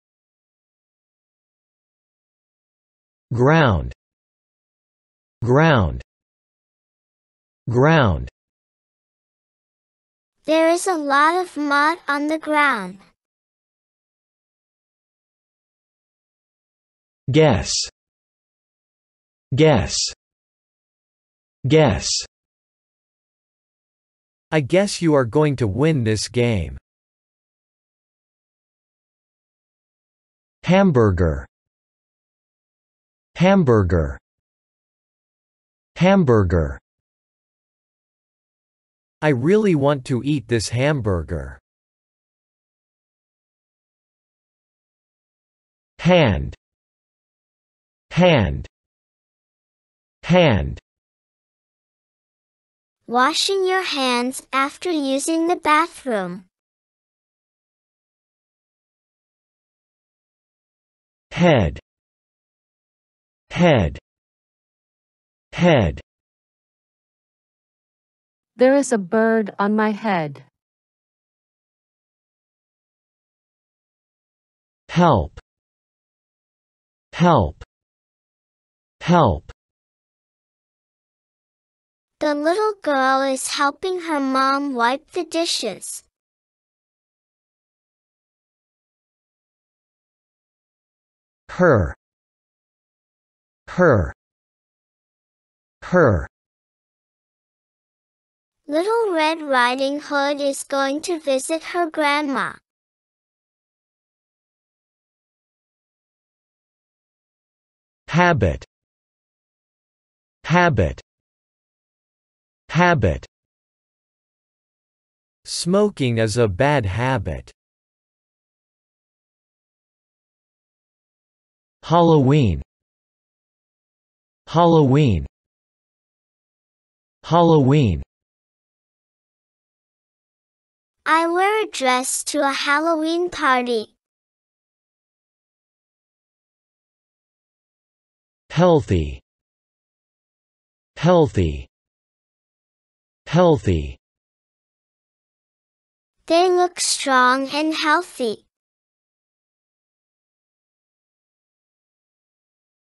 18_宜蘭縣English Easy Go (EEG) 400單字發音和例句_171-180
400 basic words with example sentences
宜蘭縣English Easy Go (EEG) 400單字發音和例句，每集共10個單字，單字分色提醒拼讀順序，提示線提醒發音規則，中英對照，真人單字與例句發音，圖文對照增加學生印象！